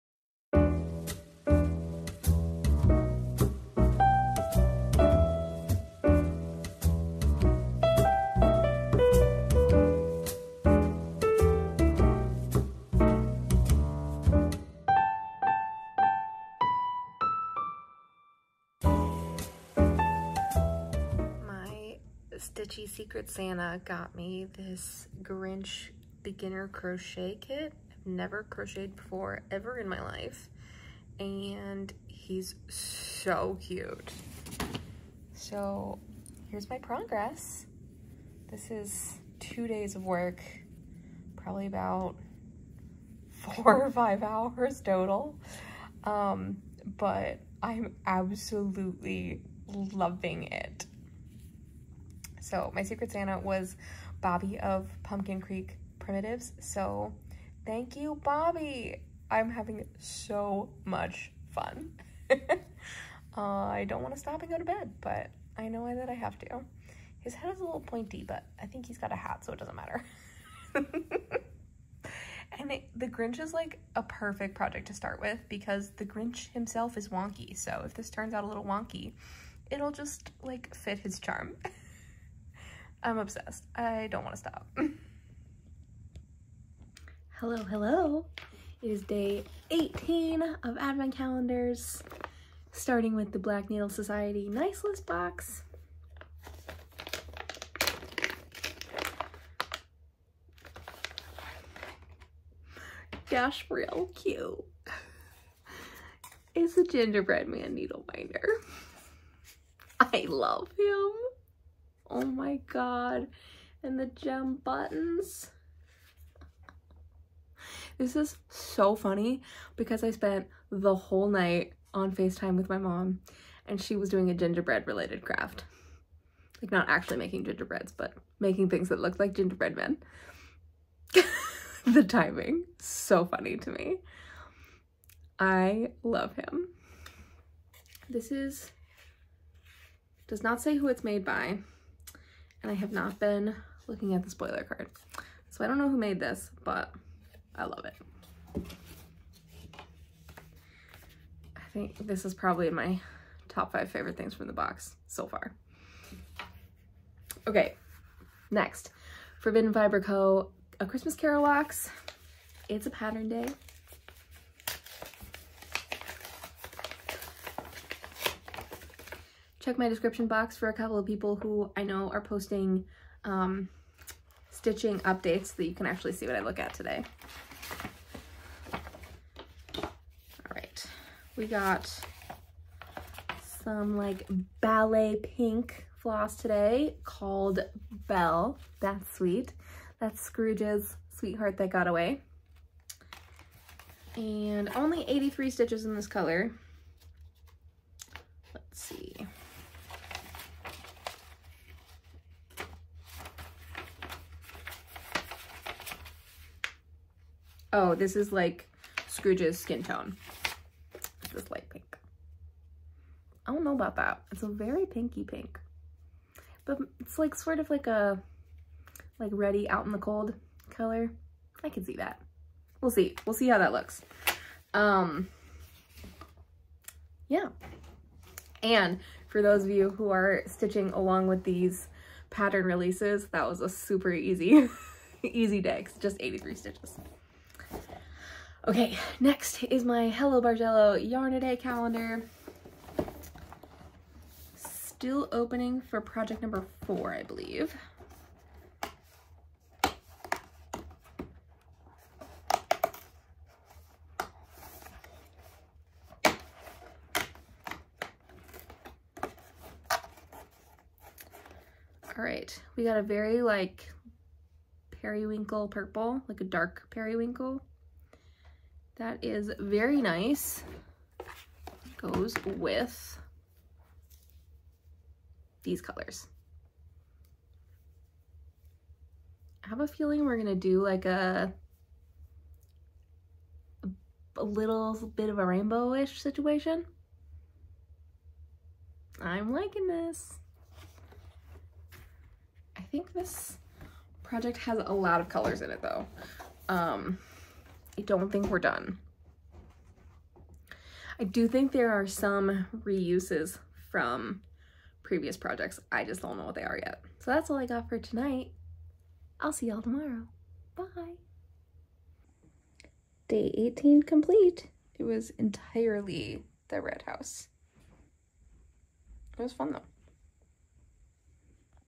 Flossmas 2023: Day 18
Advent boxes:
Black Needle Society Nice List Box
Forbidden Fiber Co A Christmas Carol Box
Hello Bargello Yarn-a-day Box
Nativity Advent Puzzle

If you want to see the Forbidden Fiber Co pattern being stitched in real time, check out  @lynnxstitchescreates  and  @coffeewinestitchingtime7085